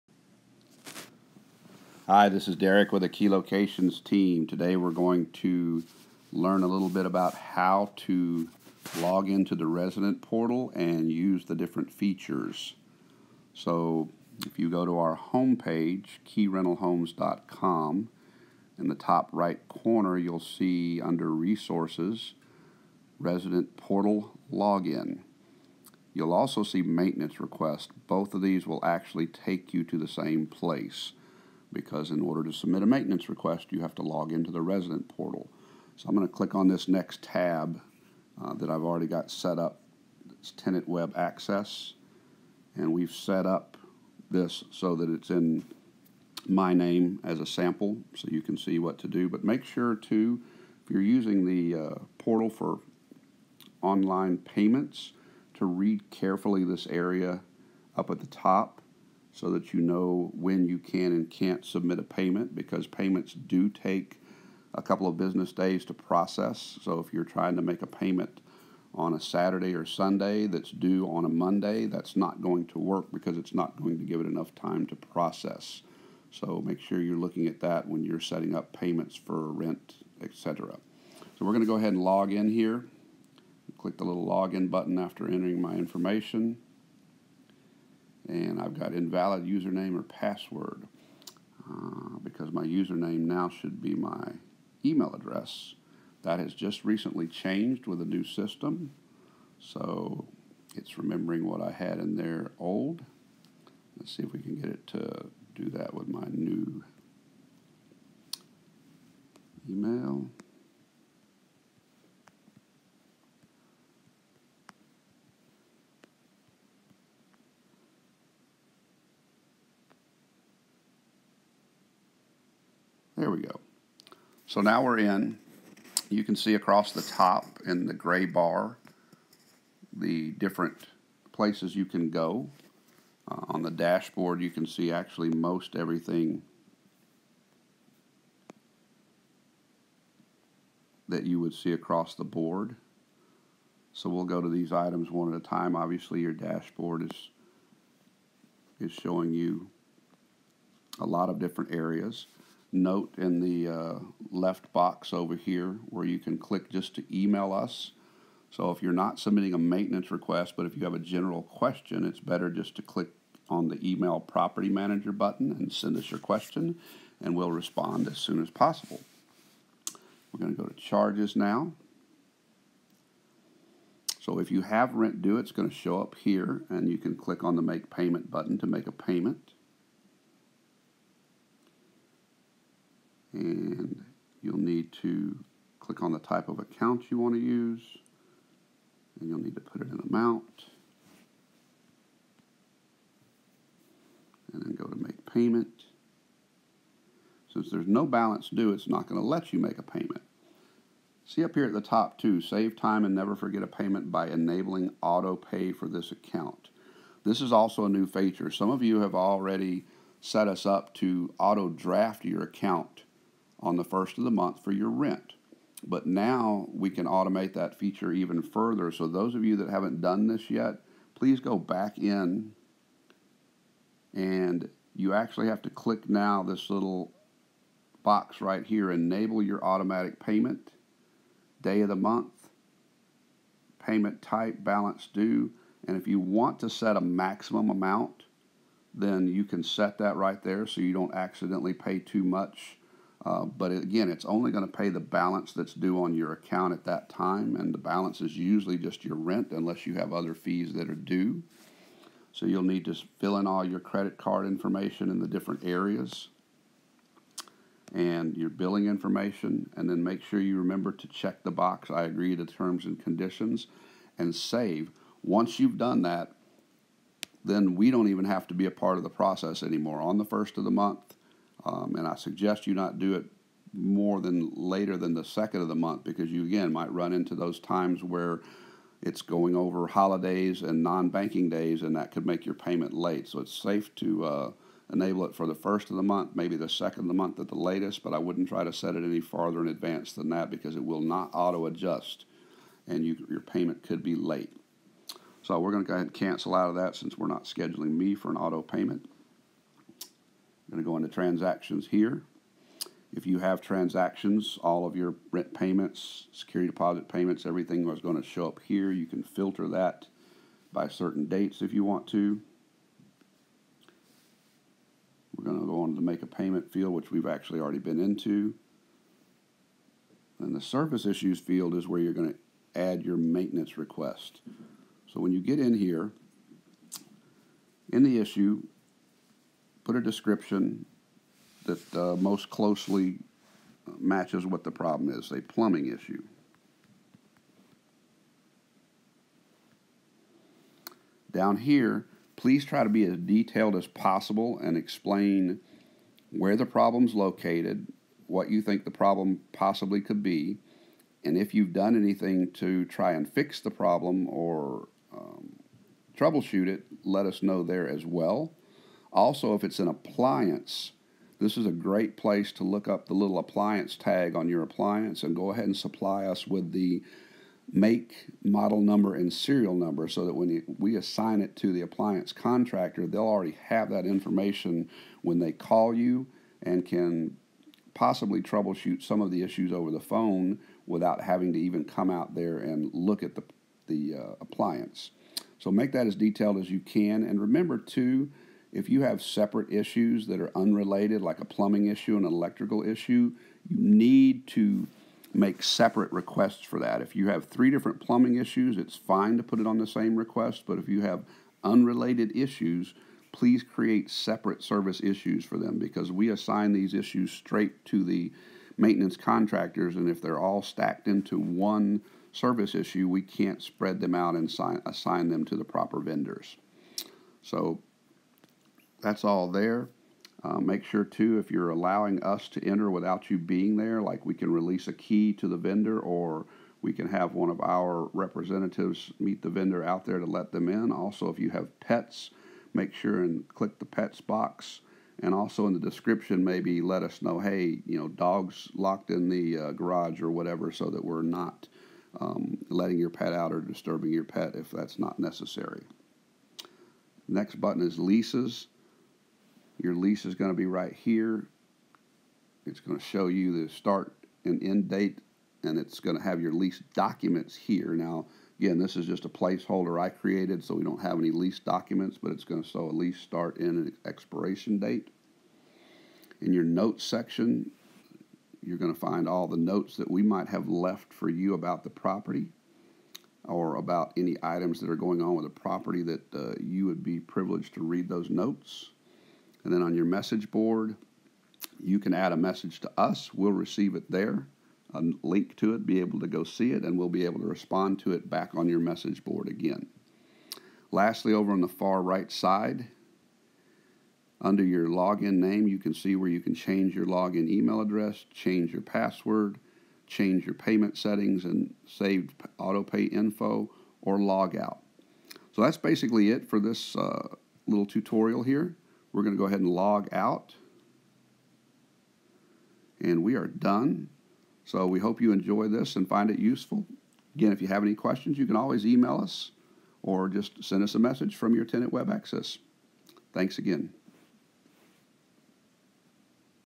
KLPM Rent Manager Resident Web Access ( Tenant Portal ) Tutorial
This is a short walk through tutorial on how to use the resident web access or tenant portal to view your account, submit maintenance requests and schedule payments etc.